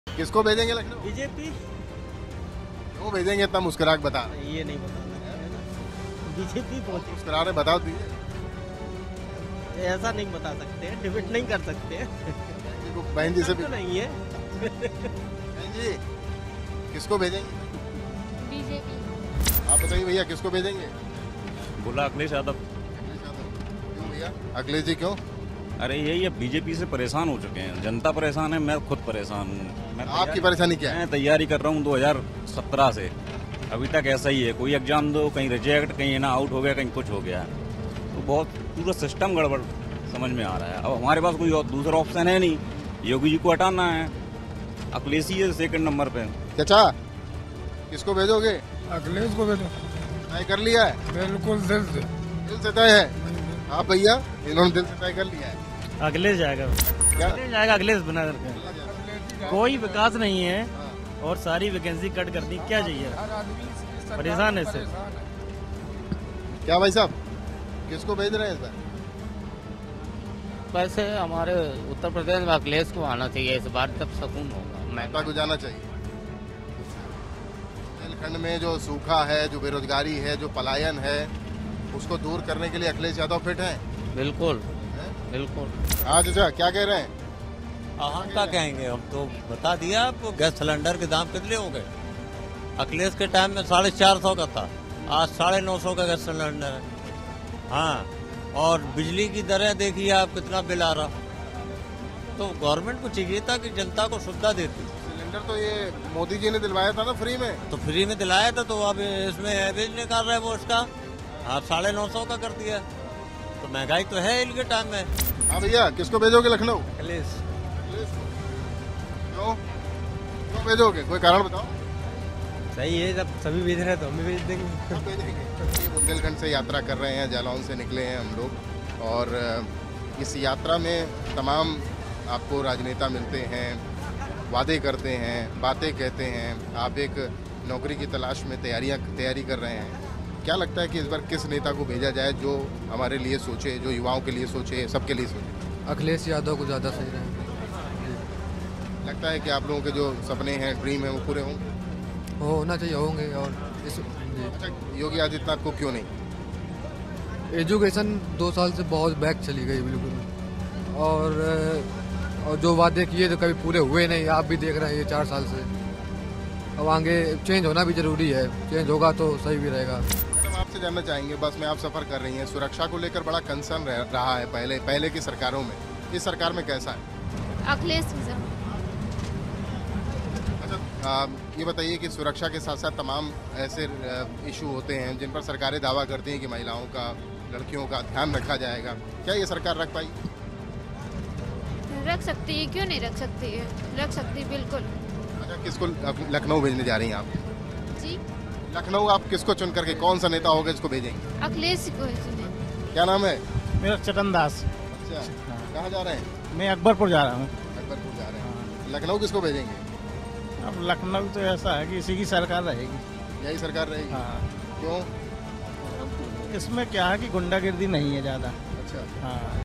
मुस्कुराते हुए बोले BJP को ही भेजेंगे लखनऊ, उधर से आई Akhilesh की आवाज ! | Lucknow Chalo
Lucknow Chalo: जालौन से कानपुर बस यात्रा के दौरान कुछ लोगों ने कहा कि वो इस बार भी बीजेपी को ही लखनऊ भेजने के मूड में हैं. तो कुछ लोगों की बीजेपी के खिलाफ नाराजगी भी नजर आई. उन्होंने कहा कि हम अब बीजेपी से परेशान हो चुके हैं.